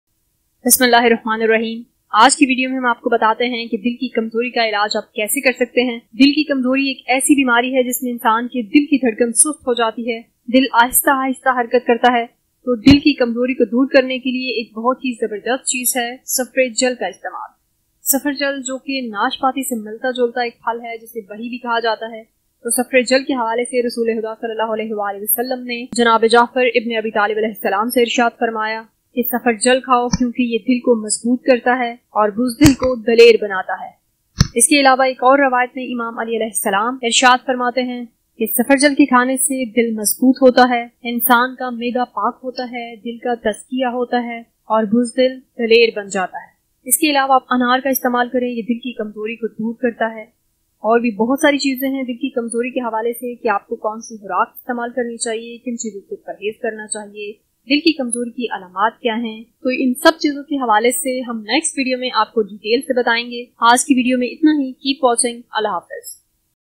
Heart weakness Treatment | Dil ki kamzori ka ilaj kaisy karein?
This video is about heart weakness. aghr kisi ka dil kamzor hai to ye b aik bimari hai iska ilaj zrori hai. aghr insan ka dil bemar ho to pora jism mutasir hota hai q k dil pori body mein main krdar ada krta hai. is video mein btaya giya hai k ap quince ka istamal kerin jissy ap k dil ki kamzori door ho jay ghi.  mazeed detail next video mein btae jay ghi .thanks